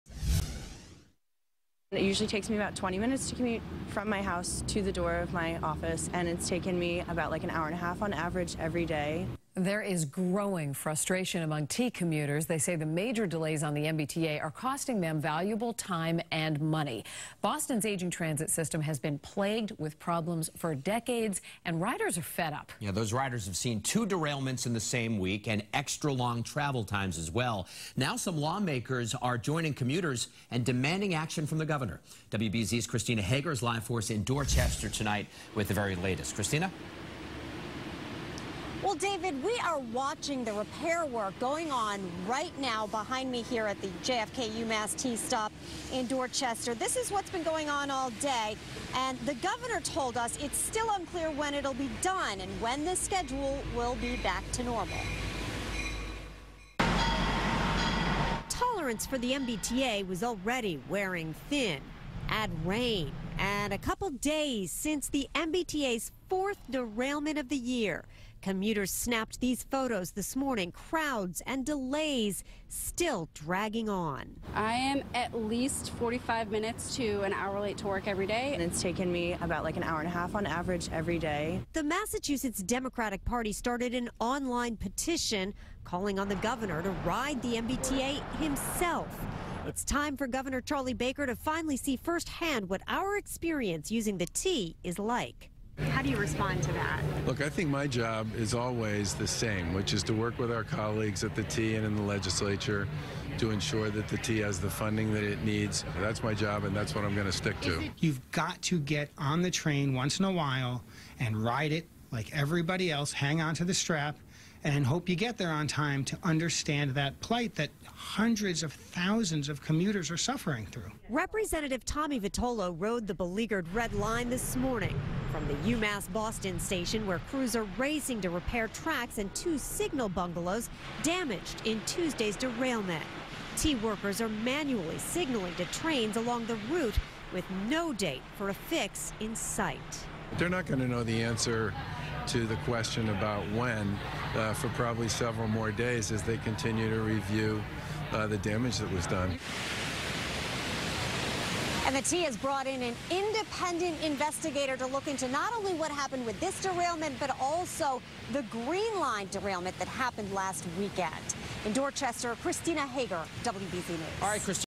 Democrats Want Charlie Baker To Ride T As Red Line Repairs Cause Delays
WBZ's Christina Hager reports.